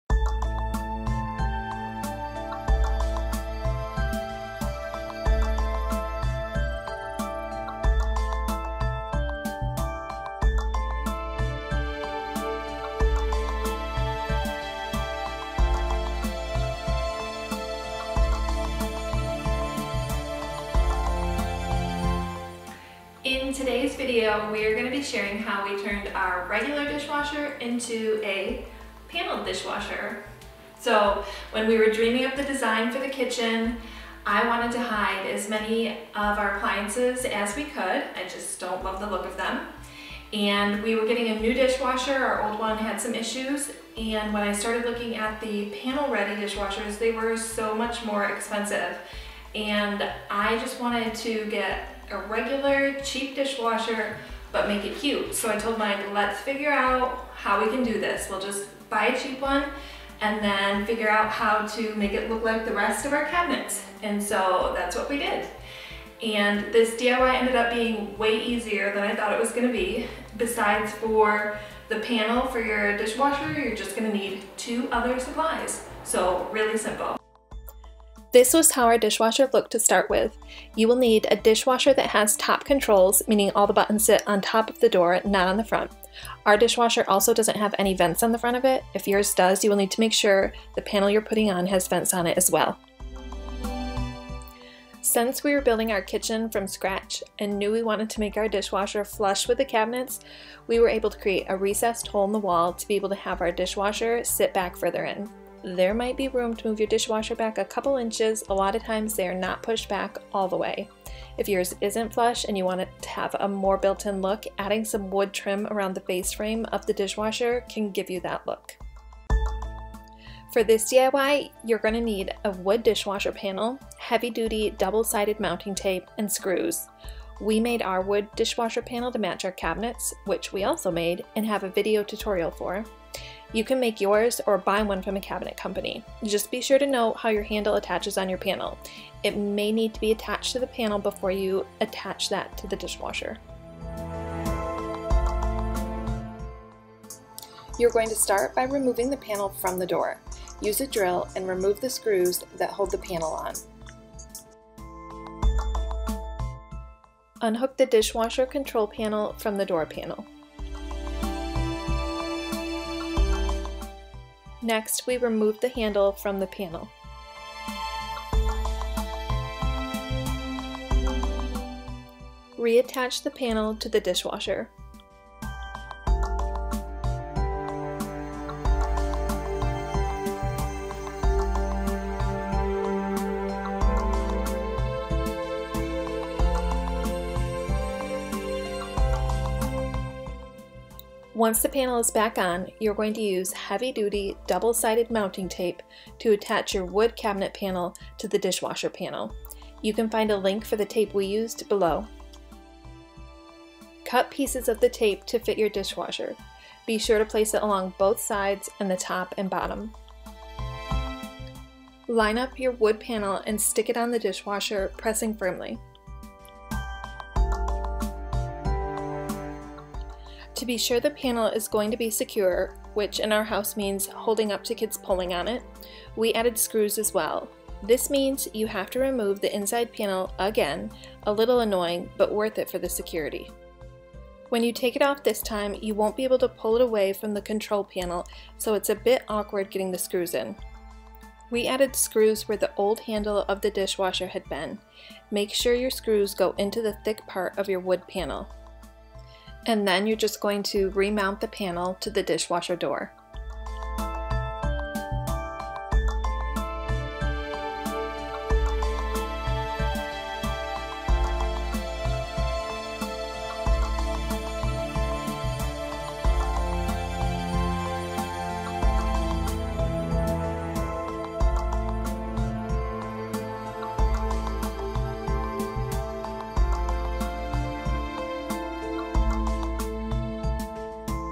DIY Dishwasher Panel - How to Put a Panel on Any Dishwasher
We're sharing how we put a wood panel on our regular dishwasher to make it look like our cabinets. No expensive, panel-ready dishwasher needed!

This is the dishwasher we have

Materials:
- wood panel for dishwasher (we made ours, see link above)
- heavy duty double sided mounting tape, you will most likely need more than one roll, we used two
- screws